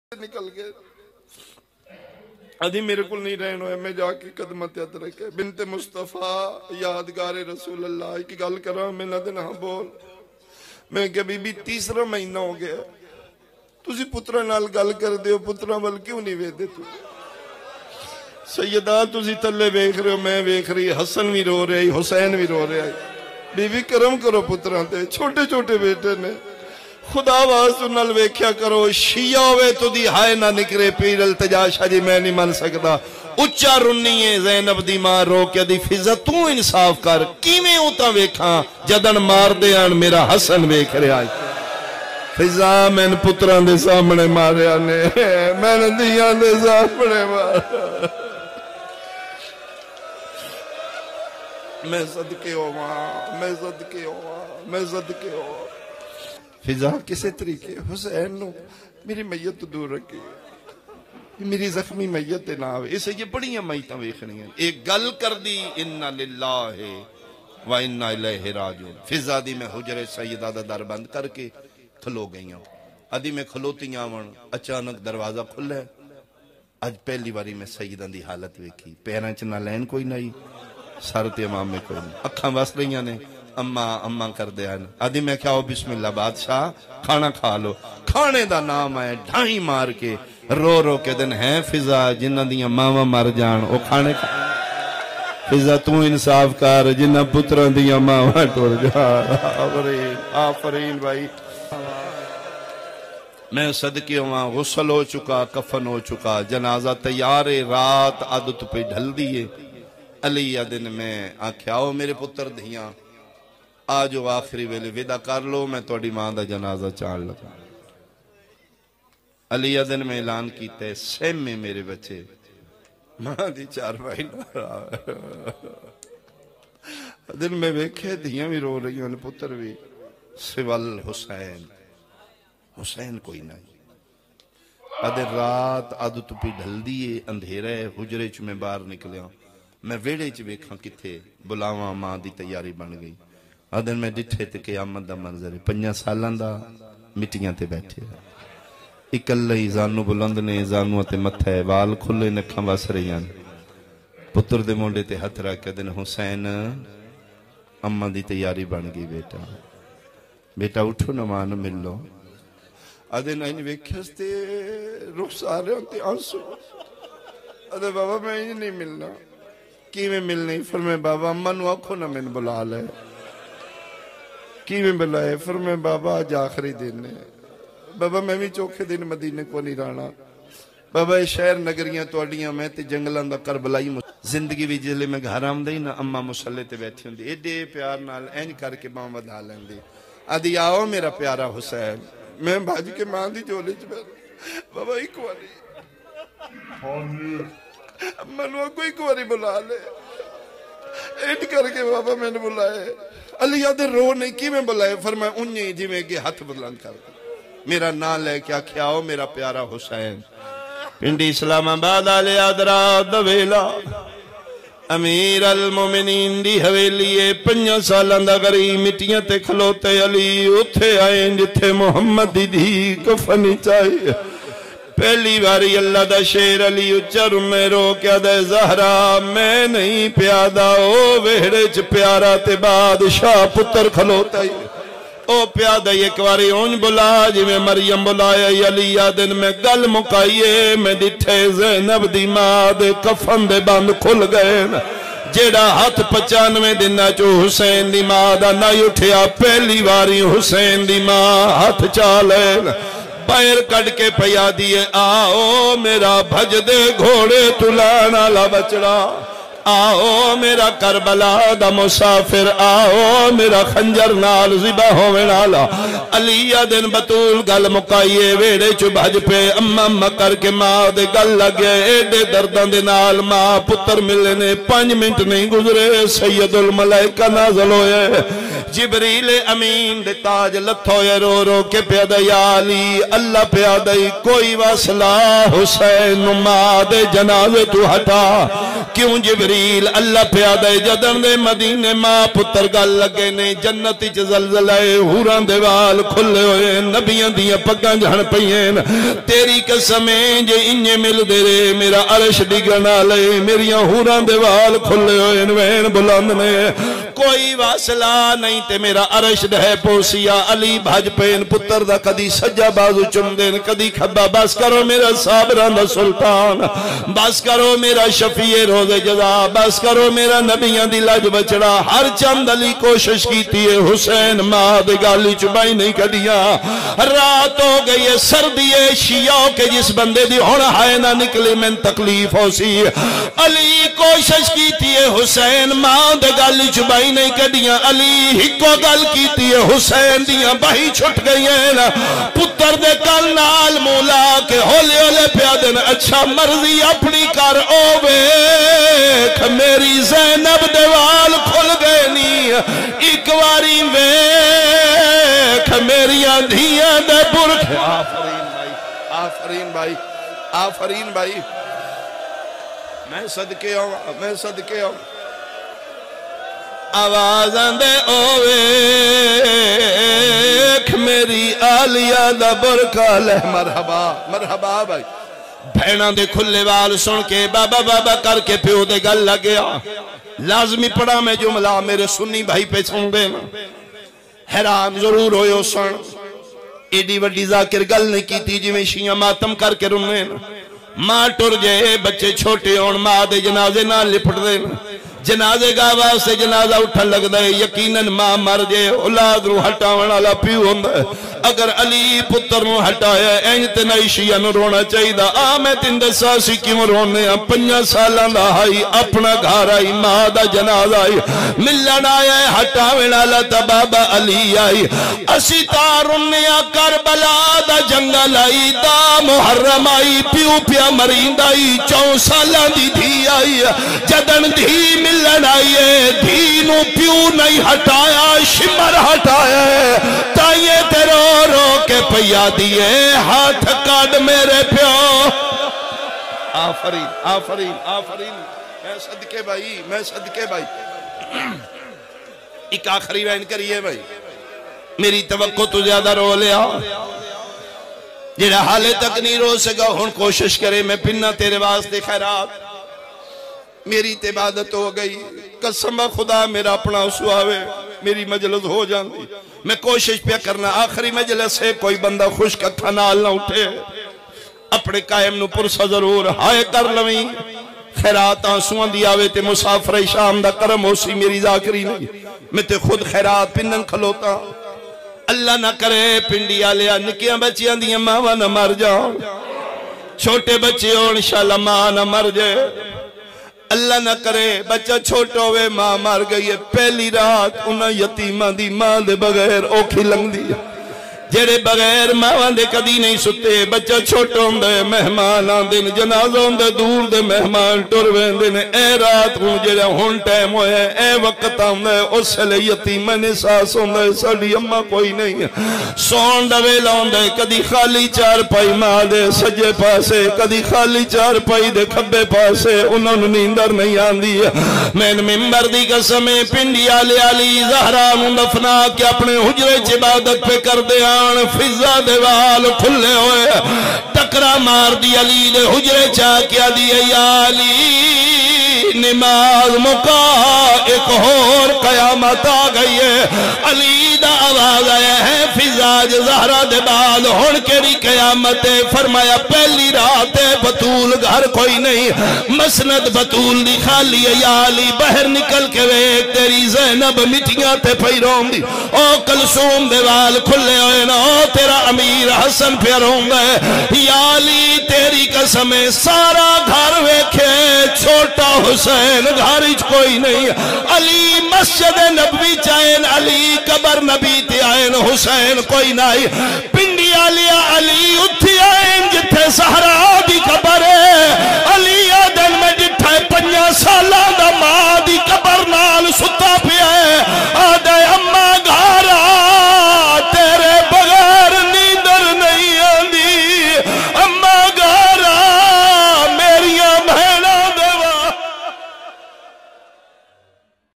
3_Jamadi-ul-Shani|Shadat BiBi Fatima(a.s)|Zakir:Syed Iqbal Hussain Shah|2022@AL_ZAIN_Production
3_Jamadi-ul-Shani|Shadat BiBi Fatima(a.s)|Zakir:Syed Iqbal Hussain Shah|2022@AL_ZAIN_Production

3_Jamadi_ul_Shani_Shadat_BiBi_Fatima(a.s)_Zakir:Syed_Iqbal_Hussain_Shah